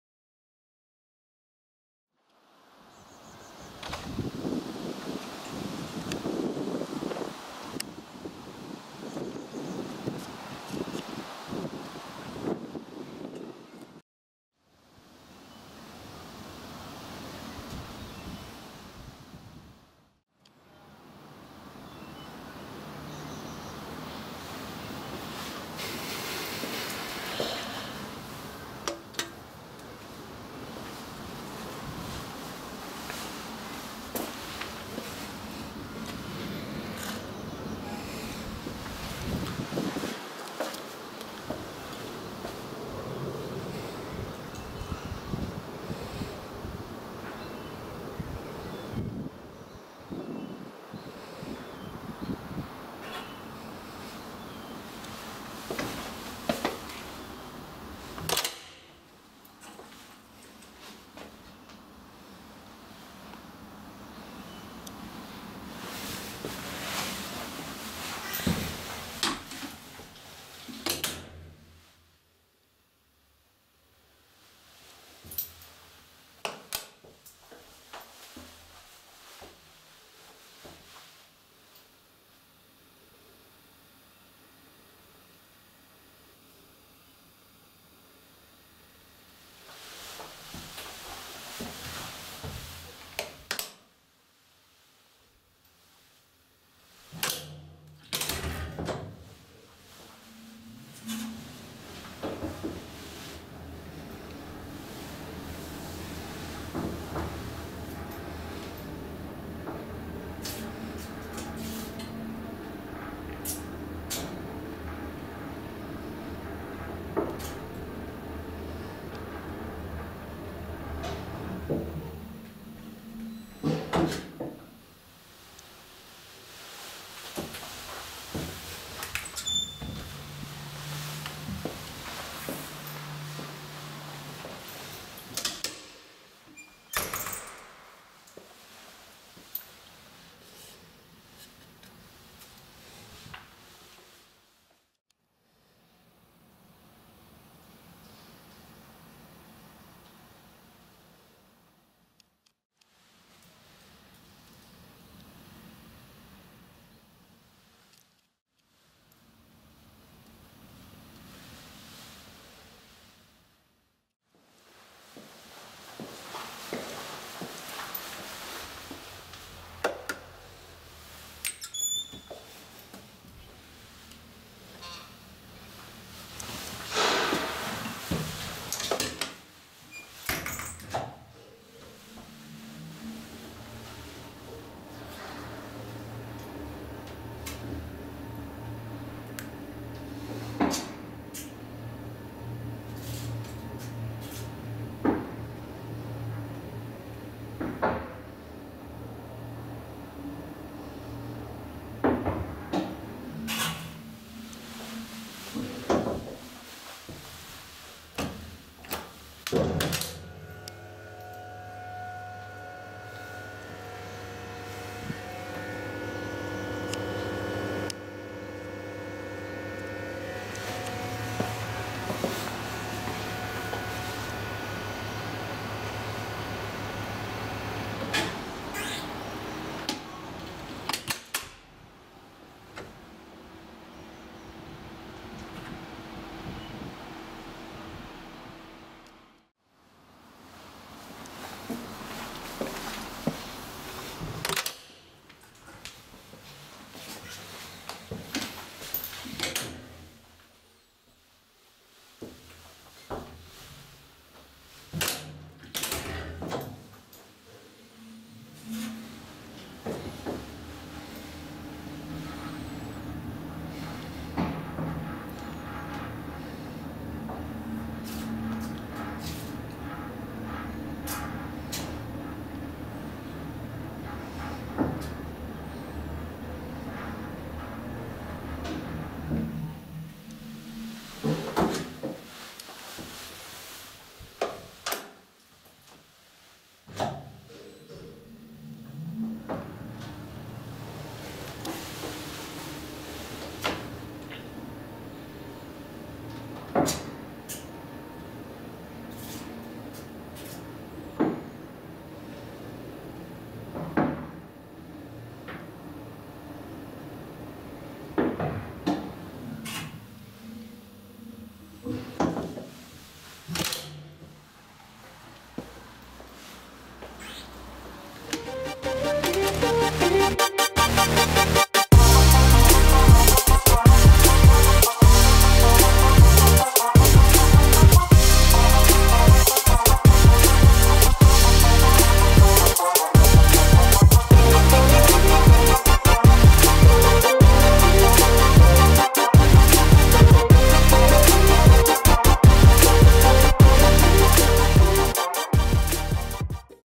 Very strange elevator setup - Skipping 5 floors!
Current moderniser: KONE
Serial: 556011
Capacity: 500Kg - 6 Persons
Floors: 2 = E ... 5

Location: Stamgatan 59, Stockholm (Östberga) SWEDEN!